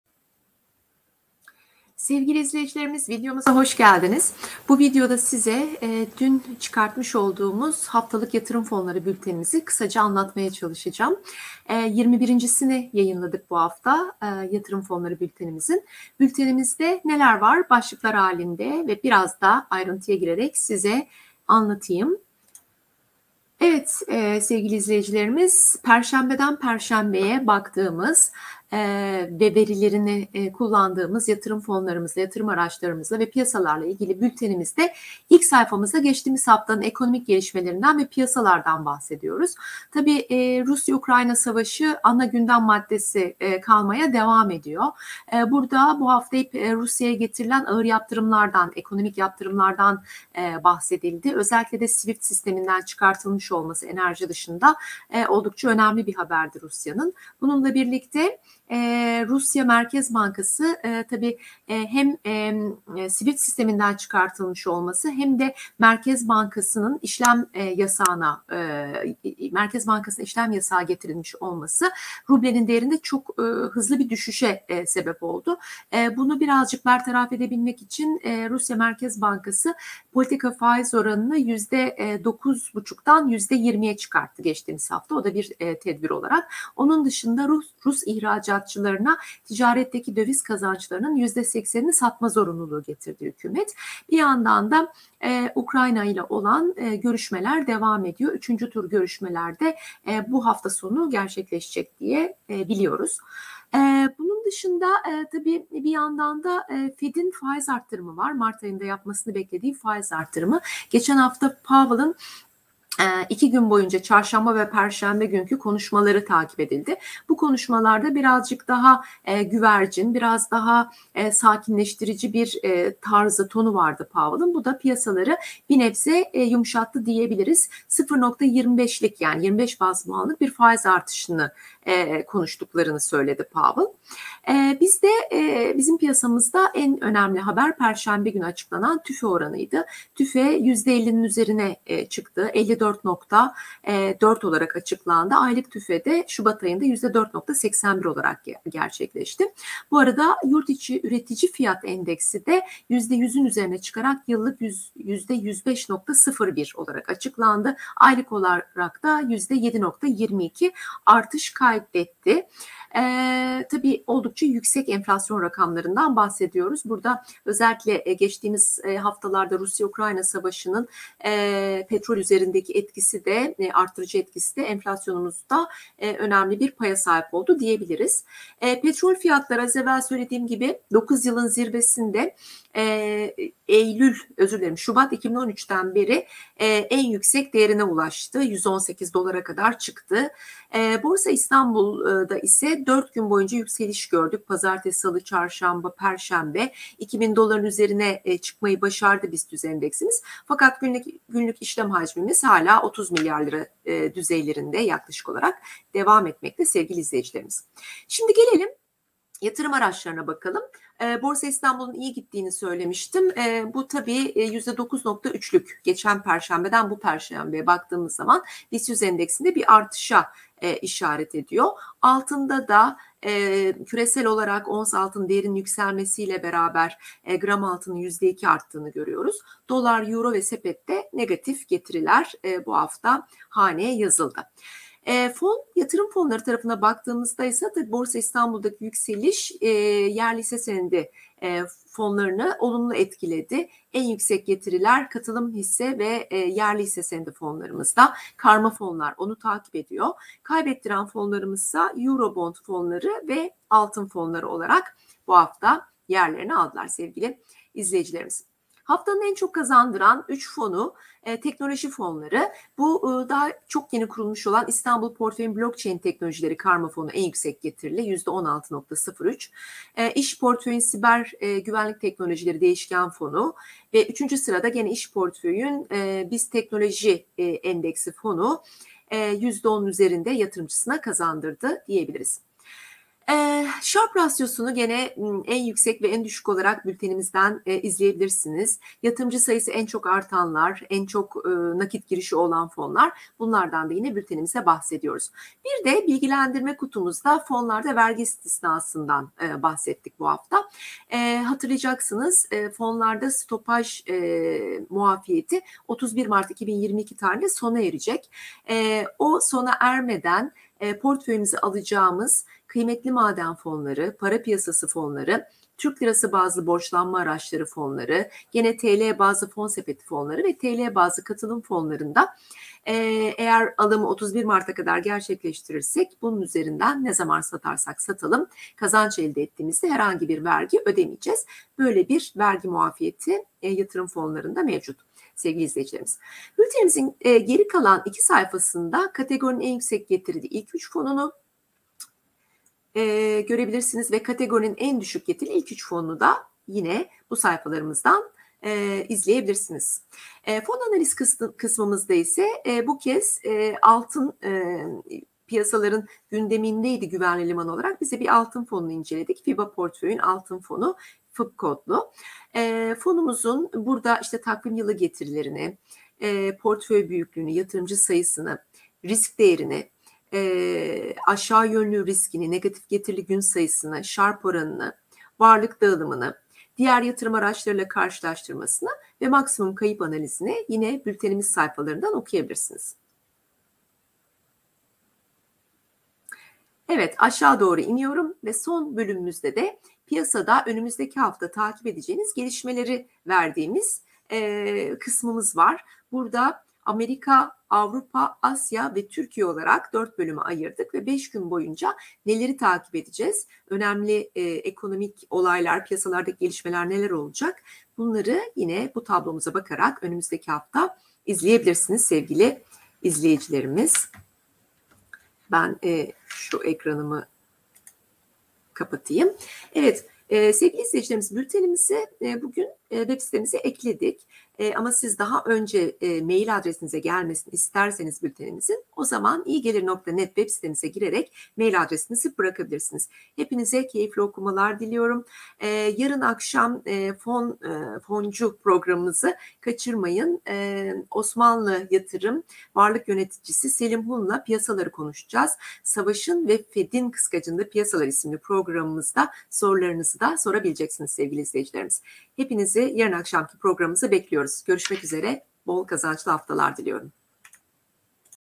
İyi Gelir Haftalık Fon Bülteni 21. Sayısı Çıktı
Yatırım Fonları Haftalık Bülteni'nin 21. sayısı çıktı!
Bu sayımızda:

- Geçtiğimiz hafta ekonomide ve piyasalarda yaşanan gelişmeleri,
- Yatırım araçlarının ve fonların getirilerini, nakit girişlerini ve yatırımcı sayılarını,
- Sharpe oranına göre fon sıralamalarını,
- Fiba Portföy Altın Fonu'nun (FIB) analizini,
- Önümüzdeki hafta piyasalarda takip edilecek gelişmeleri bulabilirsiniz.

"ABONE" olmayı ve bildirimlerin gelmesi için "ZİLİ" (🔔) açmayı unutmayın ve canlı yayınlarımızı kaçırmayın.

İleri Düzey Yatırım Fonları eğitimimizin ilkini 11 Mart 2022 Cuma 21:00-23:30 arası yapıyoruz. Sınırlı kontenjan (40 kişi) için kayıt linki: